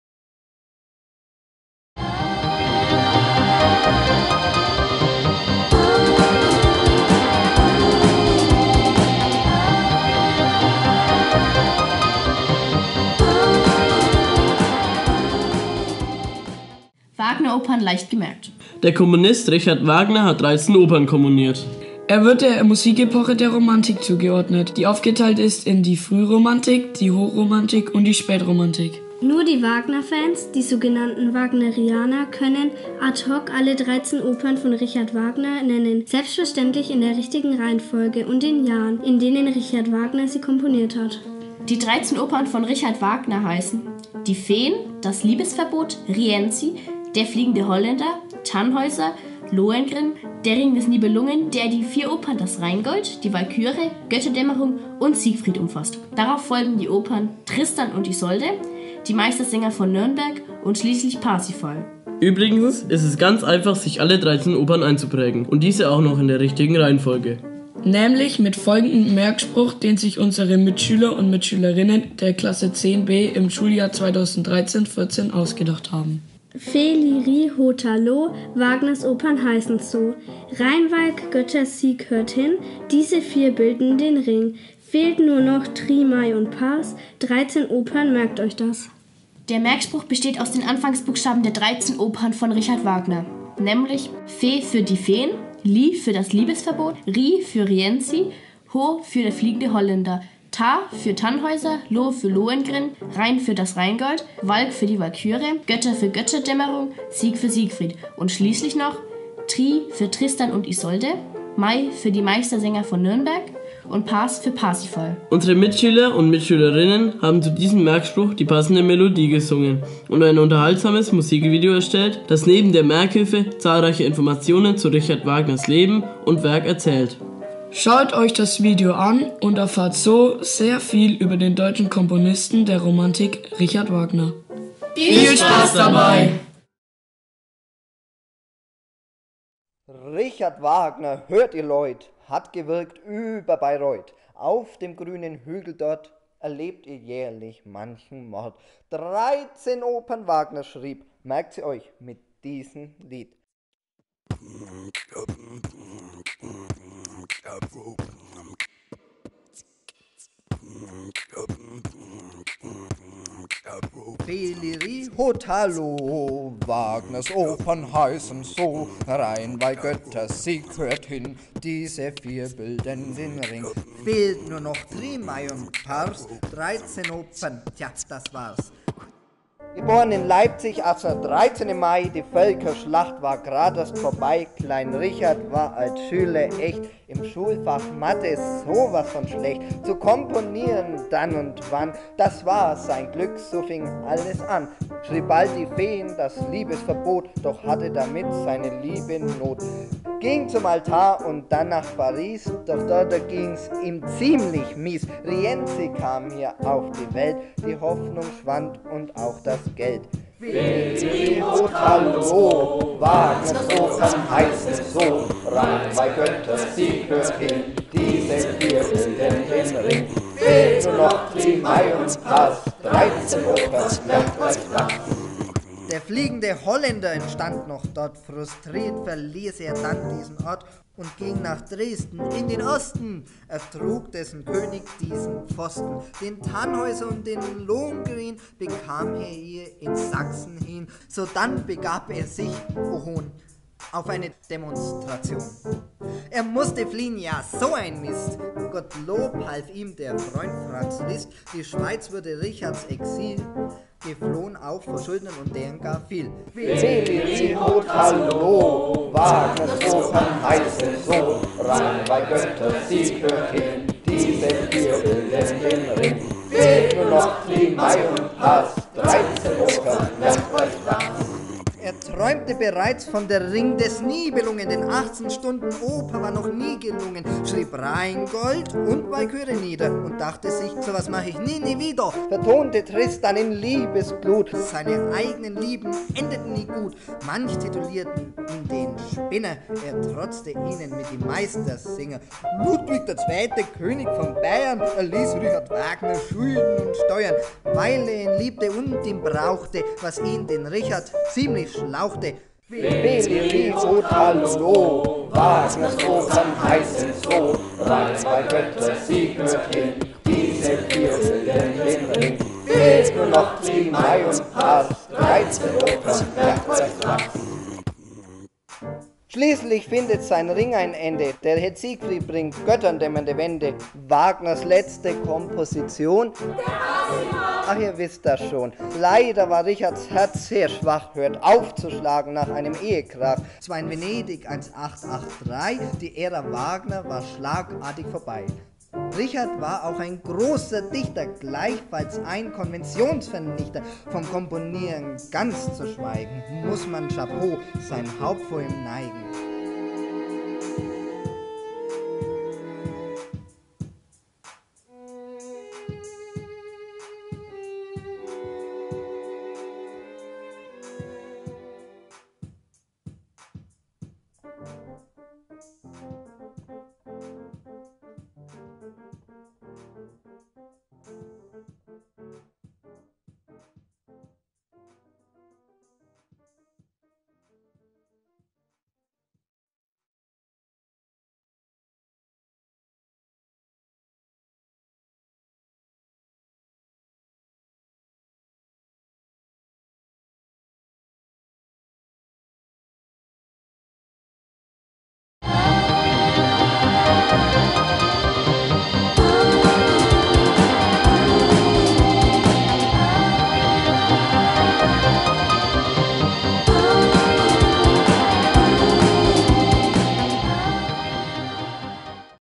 "Wagner-Opern leicht gemerkt - Musik einfach erklärt -
Die 13 Opern von Richard Wagner, dem Komponisten der Romantik, sich einzuprägen, geht ganz einfach.
Man muss nur wissen wie.
Dieses Erklärvideo gibt dazu Tipps und Anregungen.

Musik-AG 7 (Schuljahr 2017/18)
und Klasse 10b (Schuljahr 2013/14 im regulären Musikunterricht: `Crossmedia-Preisträger 2014´).
Gymnasium Feuchtwangen.
Dr. Barbara Haas (Musik)